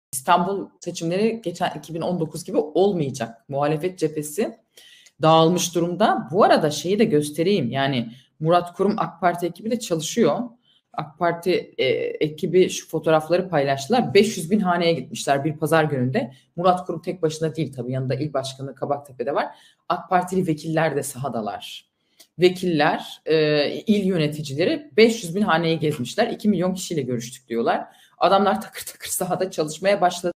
Güneş balçıkla sıvanamıyor: Nevşin Mengü'den Murat Kurum itirafı
Türkiye'de 31 Mart yerel seçimlerine yönelik geri sayım başlarken, CHP'li gazeteci Nevşin Mengü, Cumhur İttifakı İstanbul Büyükşehir Belediye Başkan adayı Murat Kurum'u övgü dolu sözlerle anlatarak, "İstanbul seçimleri 2019'daki gibi olmayacak. Murat Kurum ve AK Parti ekibi büyük bir çaba sarf ediyor." ifadelerini kullandı.